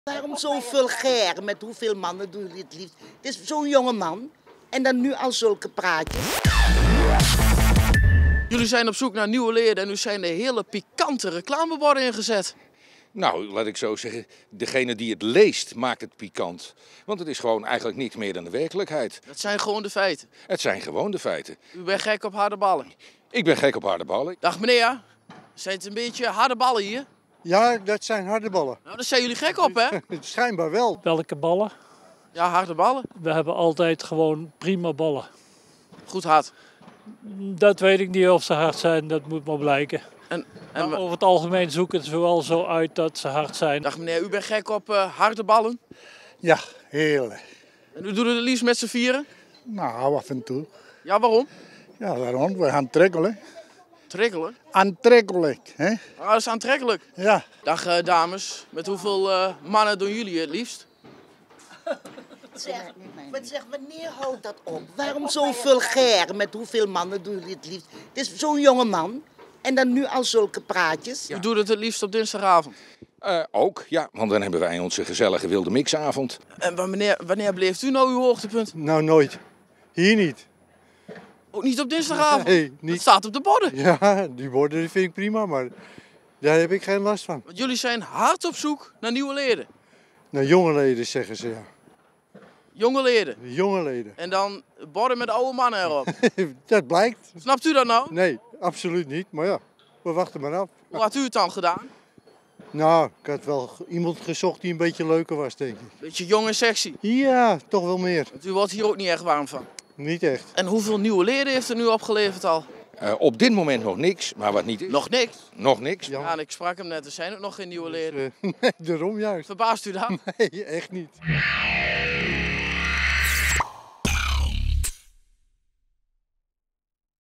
Diemen-Zuid is gek op harde ballen
Bij tennisvereniging TVDZ in Diemen doen ze het minstens drie keer per week en zijn ze niet vies van harde ballen. Zo blijkt ook uit de nieuwe wervingscampagne van de club...